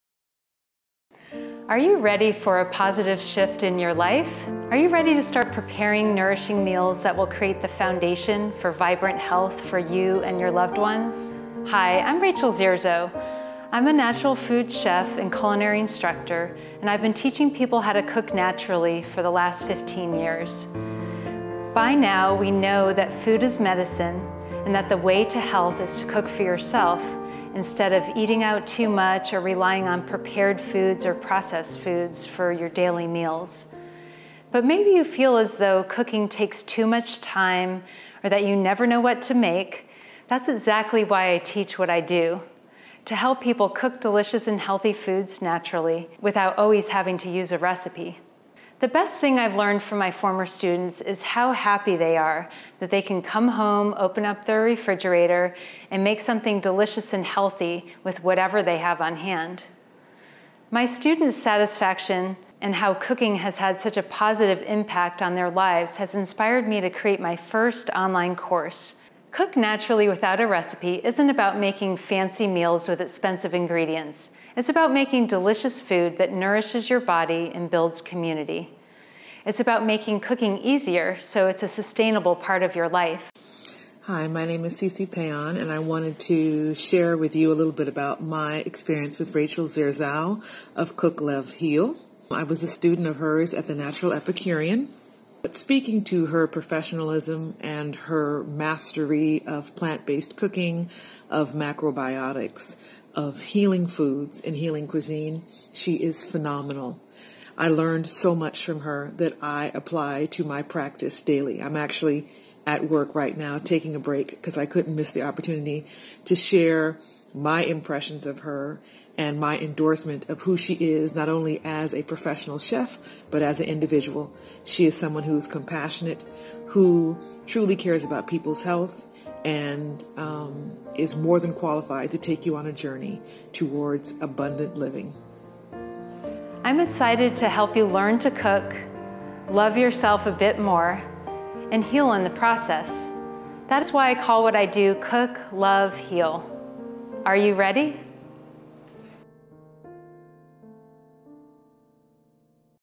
"Cook Naturally Without A Recipe with Rachel Zierzow" - Online Course Intro
Cooking for yourself is the foundation of vibrant health. Learn to create beautiful meals that nourish your body and soul. Rediscover the joy of cooking and be inspired to cook healthful meals more often at home!